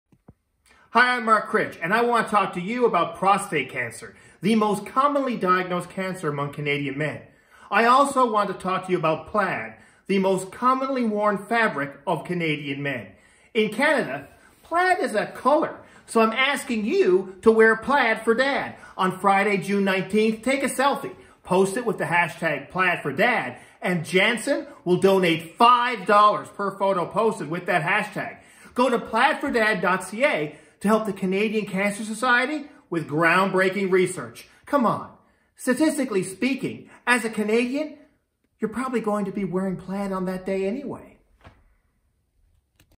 Mark Critch | PlaidforDad | Canadian Cancer Society
Join Mark Critch and honour the fathers and men in your life by registering your workplace for PlaidforDad on June 19. Wear plaid, fundraise for a great cause, and help us create a world where no one fears prostate cancer.